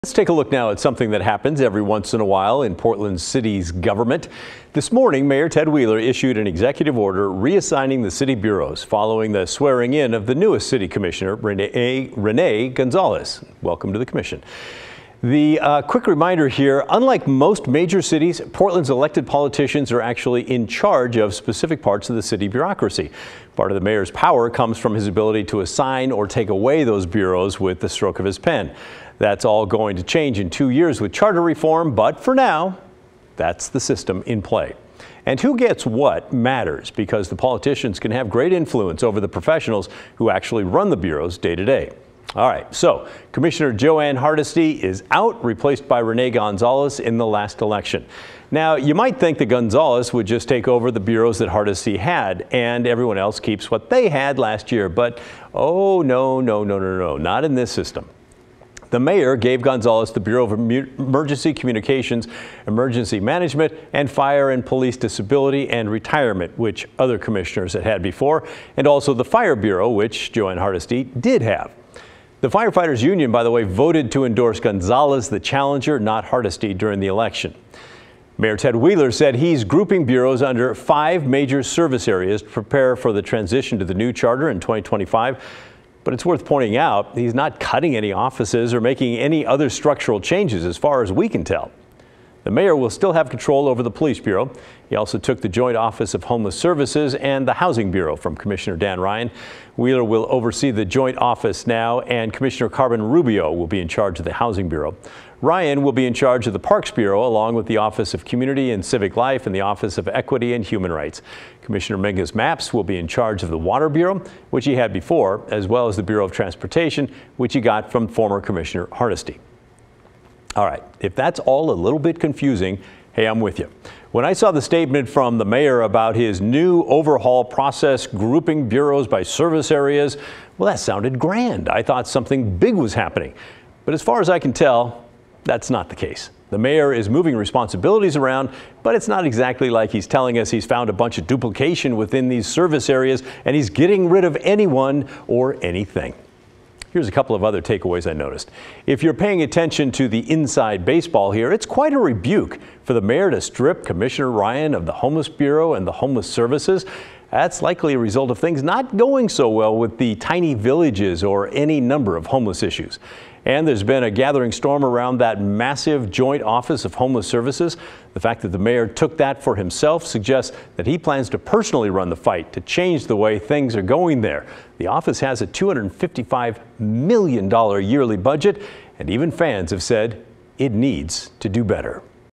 Mayor Ted Wheeler shakes up Portland city bureau assignments
With the advent of a new year and a new commissioner, as Rene Gonzalez replaces Jo Ann Hardesty, Ted Wheeler is re-shuffling which departments each commissioner will oversee. He cited the coming overhaul of Portland's system of government.